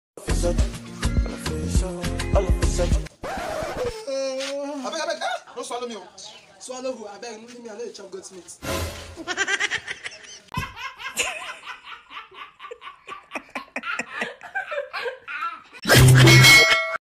Goat meat 🍗 😂😂😂 (house of shell comedy)
A funny day in Nigeria

Featuring
MC musa